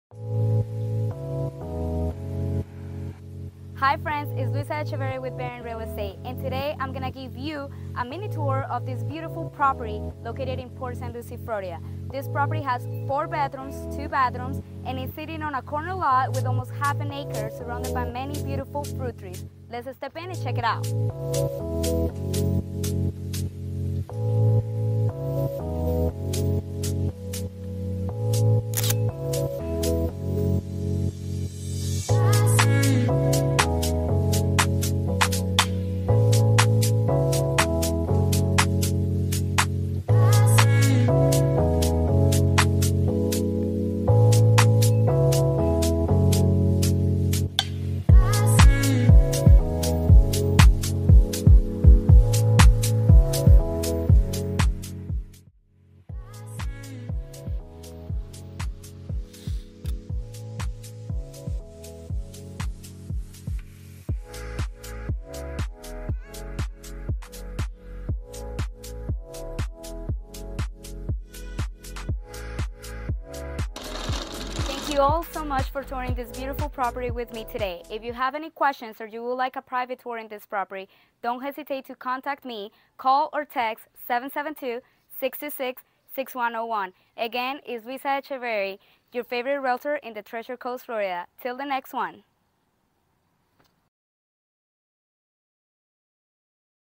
HOME TOUR - STEP INSIDE THIS GORGEOUS DOUBLE LOT HOME - 3608 SW BALLWEG ST, PORT ST LUCIE, FL 34953
L I S T E D 🔑  📍3608 SW BALLWEG ST
Step in, in this gorgeous double lot, 4 Bedroom , 2 Bathroom and a 2 car garage home!

Featuring:
‣ Formal Dining
‣ Stailnless still appliances + new fridge
‣ Sprinkler system
‣ Remodeled Bathroom (glass door is not showed on pictures)
‣ Many different kind of fruit trees
‣ New stain resistant carpet
‣ 2021 A/C
‣ 2018 Roof
‣ 0.48 ACRES!
‣ Close to both I-95 & Turnpike
‣ SPlenty of space for a pool!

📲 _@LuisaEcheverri
BaronReal Estate